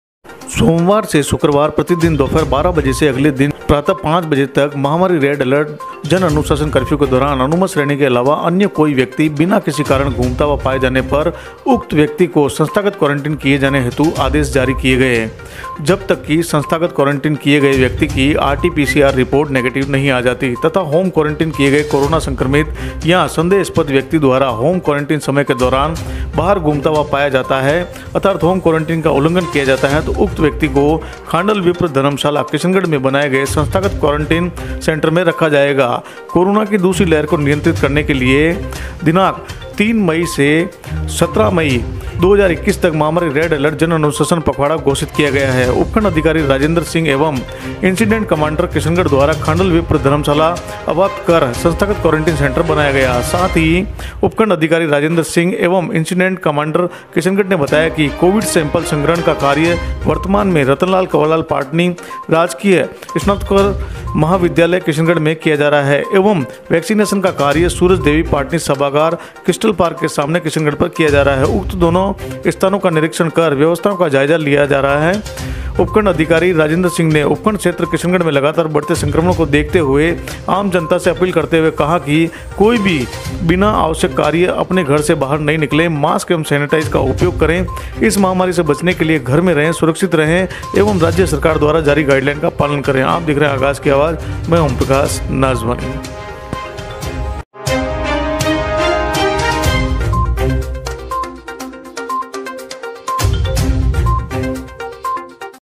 बिना कारण घूमने वालो को प्रशासन करेगा 1-14 दिन कोरन्टीन, RTPCRनेगेटिव आने पर ही दूसरे दिन छोड़ा जाएगा
बिना कारण घूमने वालो को प्रशासन करेगा 1-14 दिन खण्डल विप्र धर्मशाला में कोरन्टीन, RTPCR नेगेटिव आने पर ही दूसरे दिन छोड़ा जाएगा